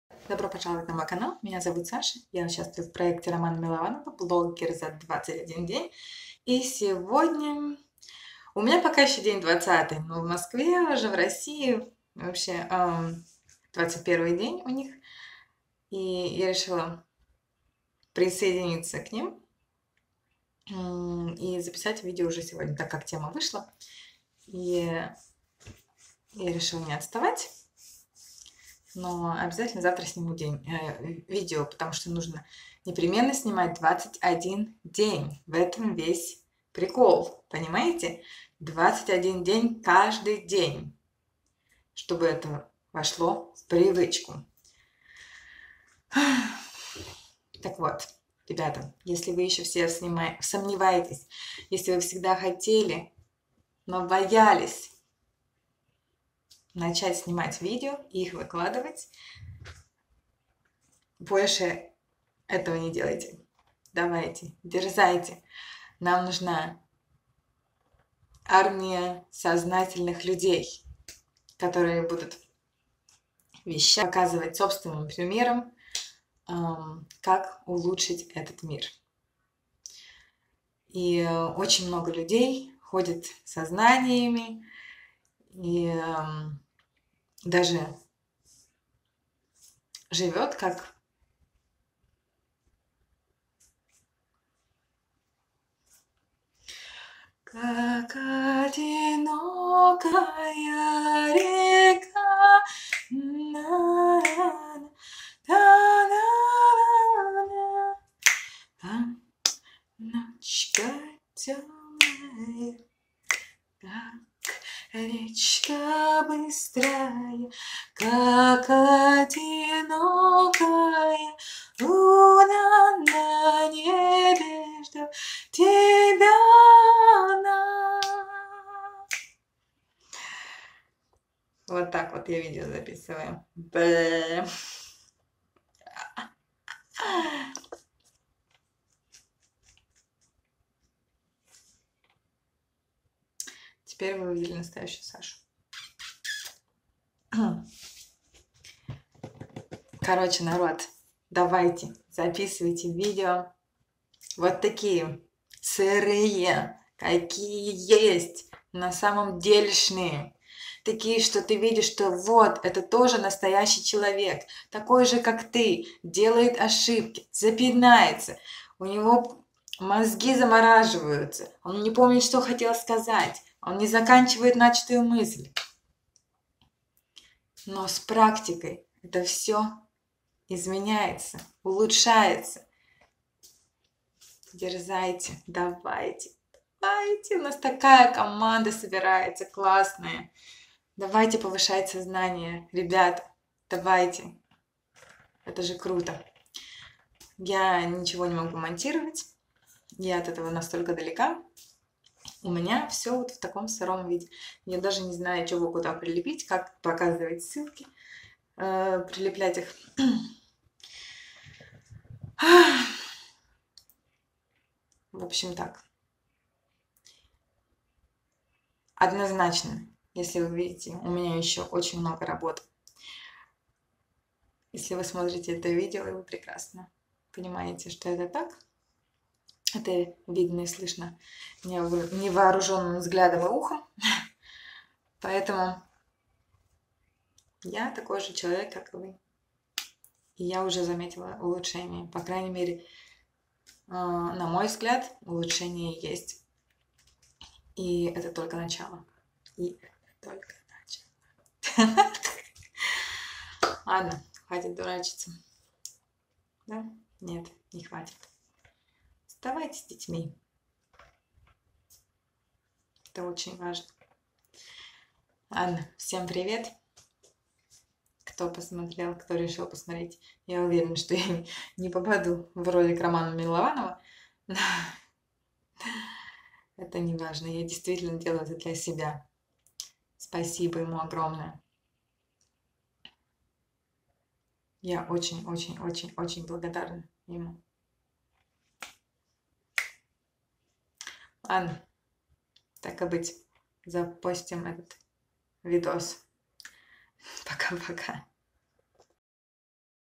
Блогер за 21 день.День 21. Мотивация.
В общем и целом я добилась чего хотела за этот проект - наконец могу быть собой на камере и ещё и без стеснения вот так взять и залить видео которое не получилось на мой взгляд. Всего пару раз я выкладывала 2-й дубль, т.е. в 90% за весь челлендж все видео были сняты за один дубль, как есть.

Огромная благодарность Роману за идею и мотивацию!!!
Благодарю всех кто потратил хоть сколько-то времени на просмотр моих видео!!!

Всем участникам больших успехов!!!💟🙏💟